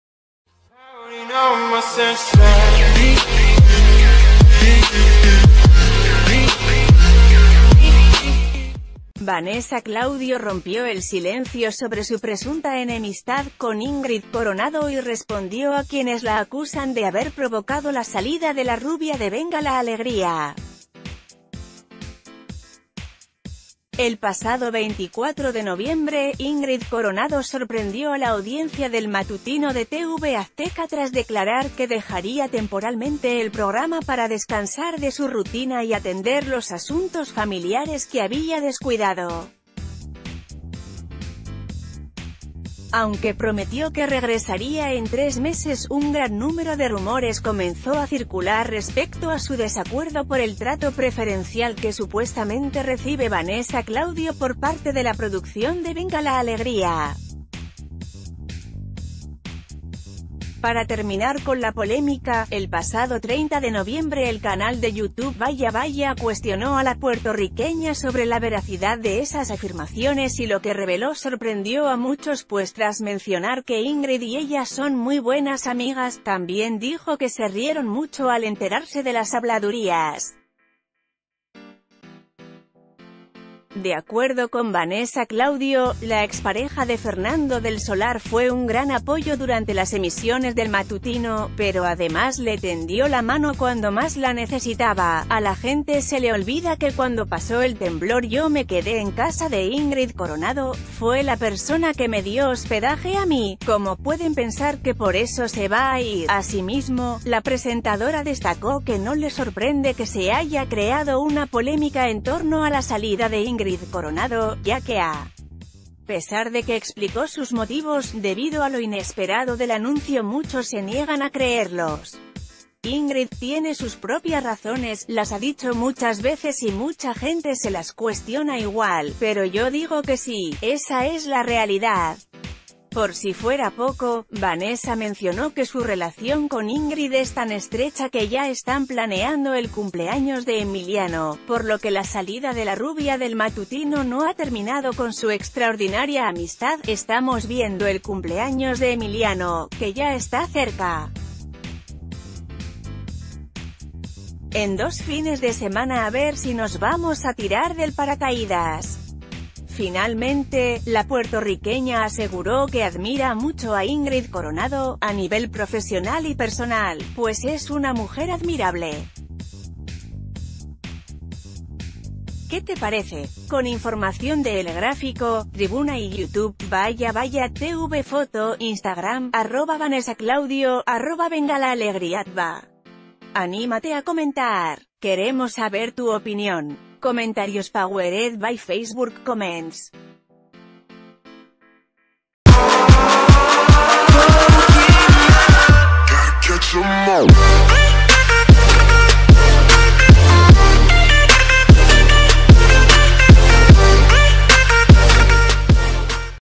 Vanessa Claudio responde a quienes la culpan por salida de Ingrid Coronado
Vanessa Claudio rompió el silencio sobre su presunta enemistad con Ingrid Coronado y respondió a quienes la acusan de haber provocado la salida de la rubia de Venga la Alegría. El pasado 24 de noviembre, Ingrid Coronado sorprendió a la audiencia del matutino de TV Azteca tras declarar que dejaría te...